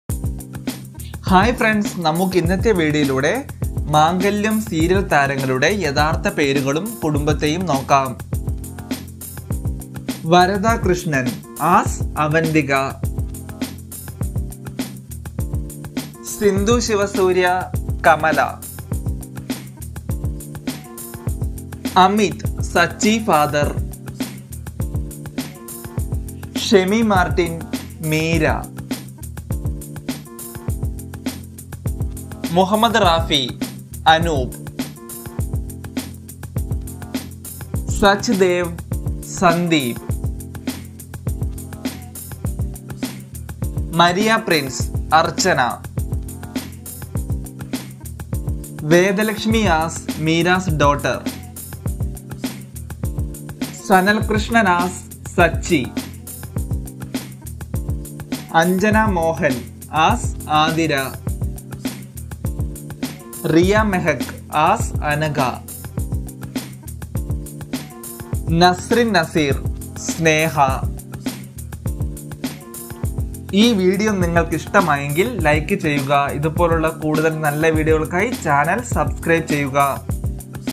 mangalyam serial actors realname and realfamily | serial cast zeekeralam | sachi |archana
this video includes real name and real family of mangalyam serial actors.